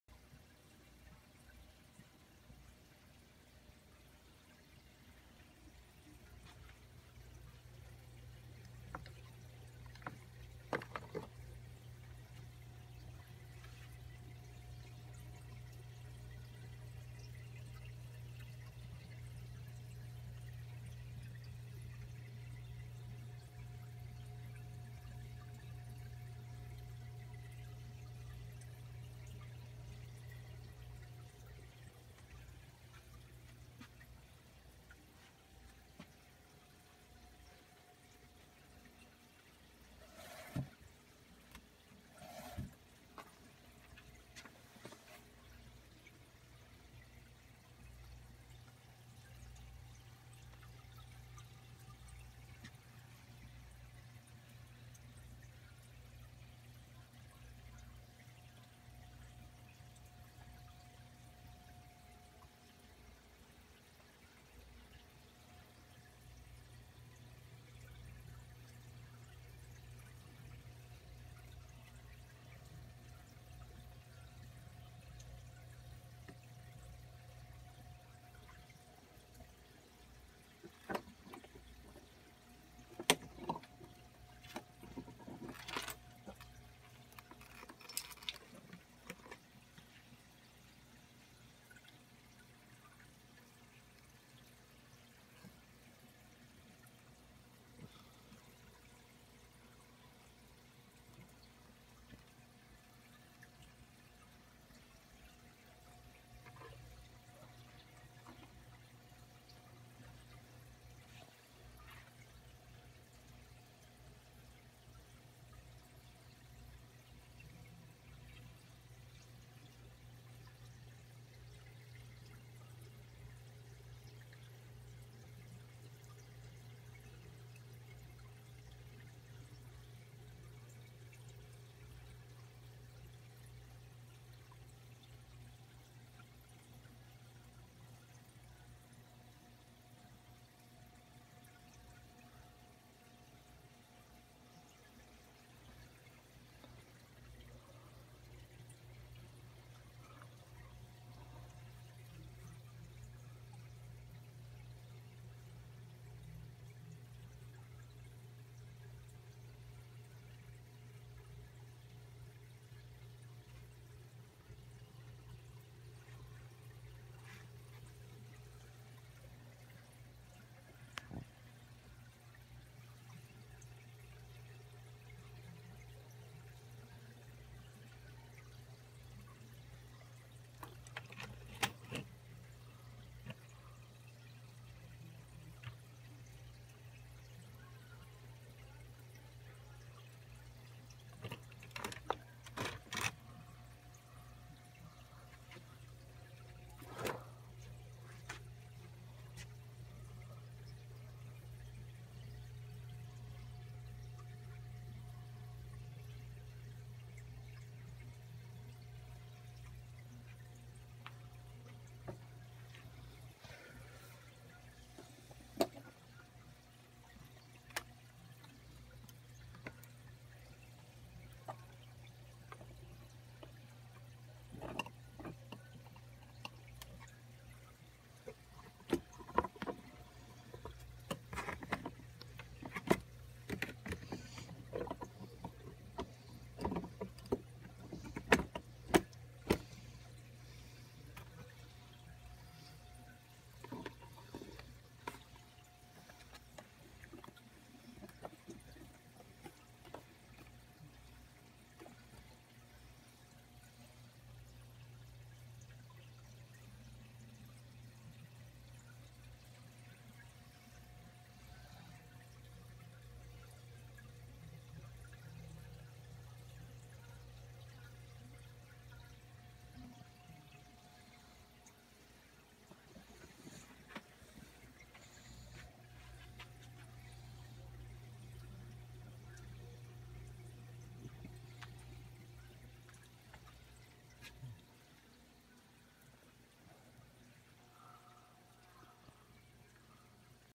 1/6 scale rc crawler new bright raptor hardbody
Custom 1/6 scale rc crawler frame from a axial scx10 deadbolt extended frame to 24" 2.2 proline wheels and rc4wd mudslinger tires new bright ford raptor hardbody more to come on next video